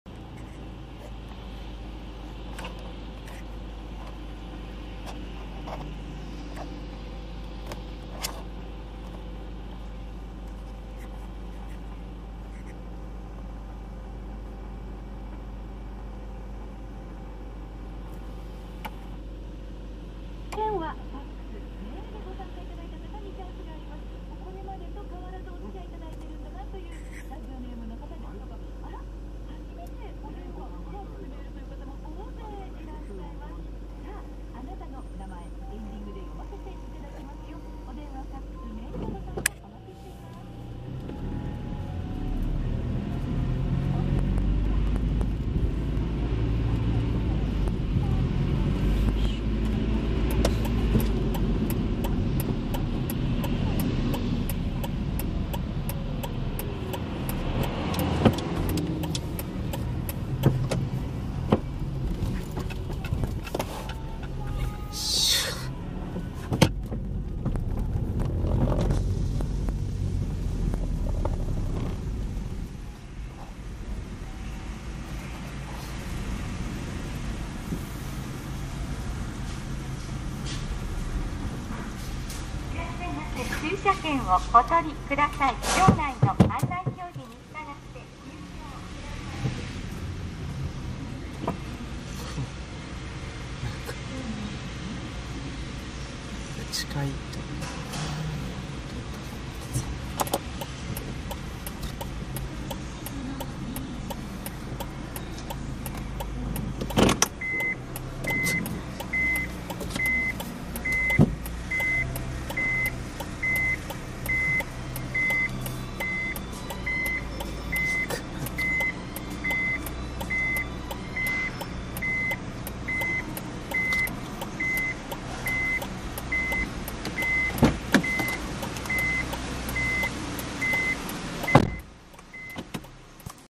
7条駐車場【駐車券発行機】：入口 [ 旭川市 360p 車載動画 2008 地下駐車場ドライブ] Asahikawa Underground parking,Hokkaido Japan
市役所利用するときに使う7条駐車場を利用するドライブです。ドライブ
時期は、2008年4月上旬。parking

0:00～0:40 6条通を直進（永隆橋通交点）
0:55 6条通から駐車場（右折）
1:15 入口ゲート（駐車券をとる）